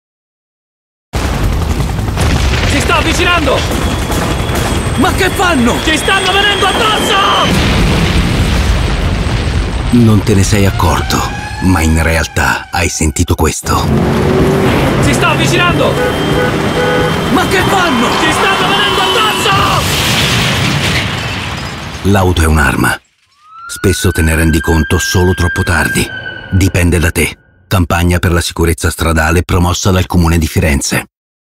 Campagna di comunicazione 2016 per combattere la Violenza Stradale
L'auto e la moto come armi ! dipende da noi ! Campagna di comunicazione per la sicurezza stradale 2016 nell'ambito del progetto DAVID